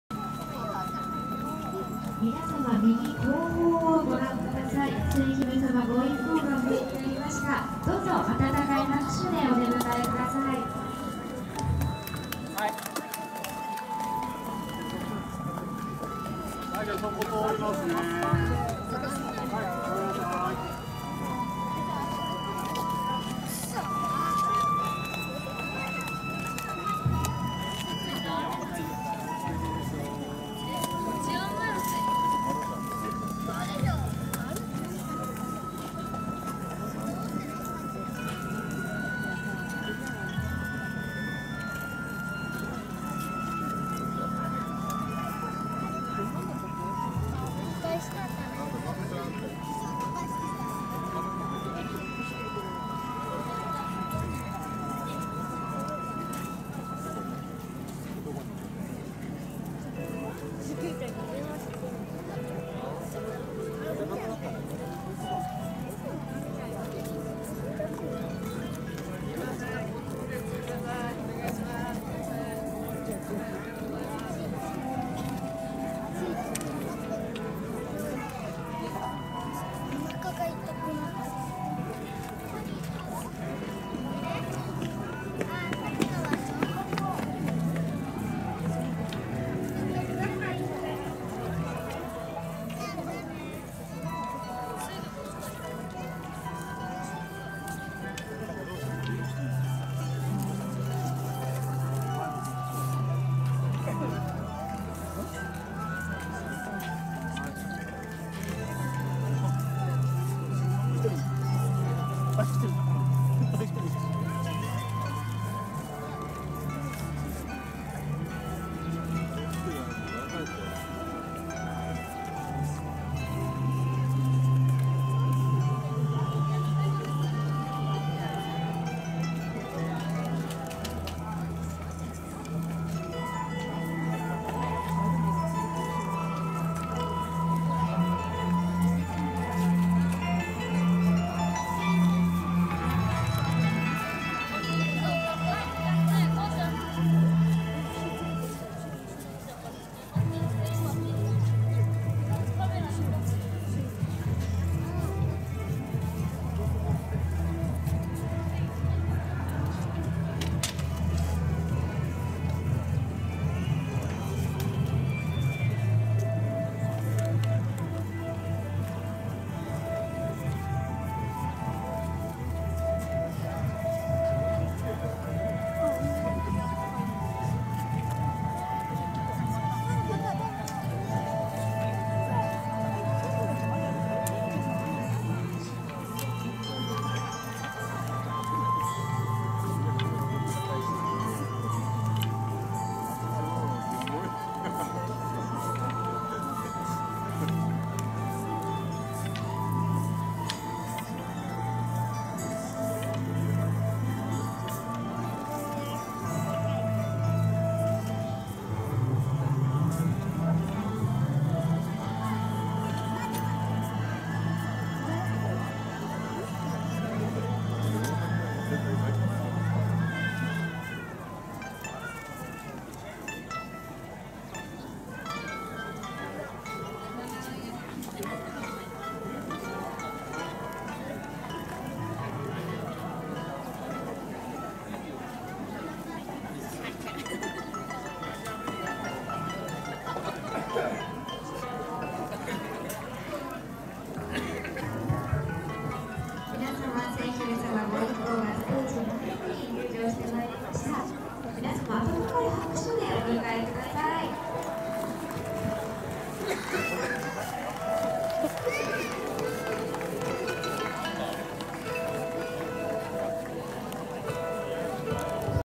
水海道千姫まつり2016 千姫さま常総ご回遊！2016/04/10
千姫さま御一行のご回遊です。旧カスミを14:00に御出立、宝町大通りを回り、市民の広場にご到着です。手を優雅に振り沿道に皆さんに応えられていました。とても素敵な千姫さまです。